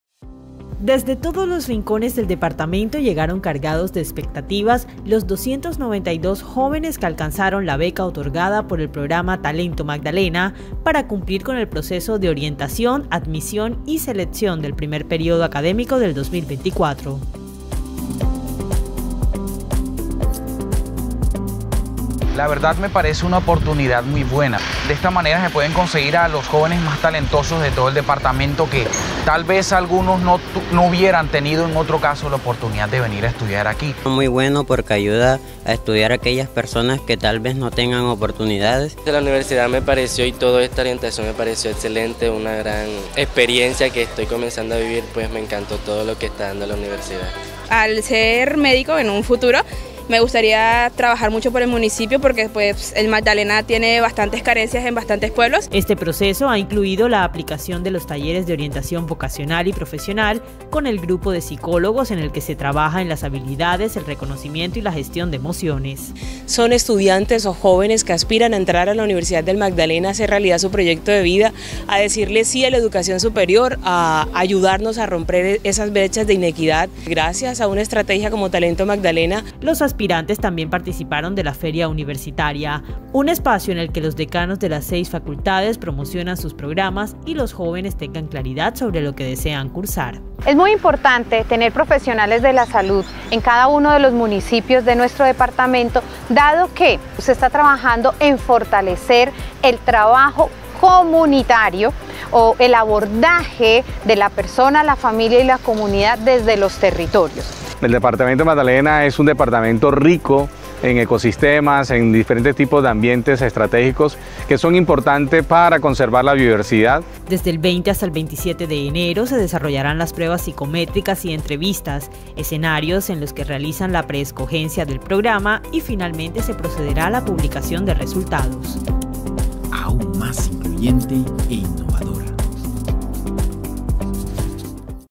292 jóvenes del Departamento cumplen con su proceso de admisión 2024-I en UNIMAGDALENA
De conformidad con el Acuerdo Académico N°26 de 2023, los aspirantes becados por el programa Talento Magdalena continúan cumpliendo con el calendario de ingreso y admisión para el período 2024-I, que avanza con el proceso de orientación, admisión y selección. El equipo de la Dirección de Desarrollo Estudiantil, Bienestar Universitario y el Grupo de Admisiones, Registro y Control recibió calurosamente a los jóvenes y sus familias, desde los diferentes municipios del Departamento.